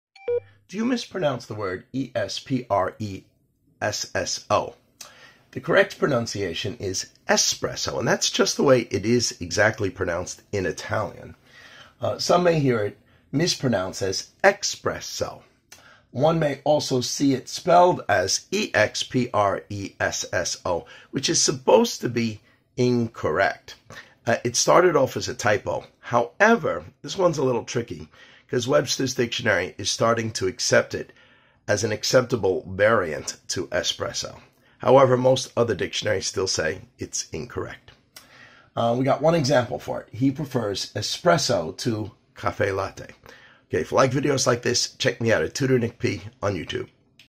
Do You Mispronounce the Word Espresso? Many Do - English Tutor Nick P
In this video, we will cover the common error of pouncing this word incorrectly, give the correct pronunciation and an example of its use.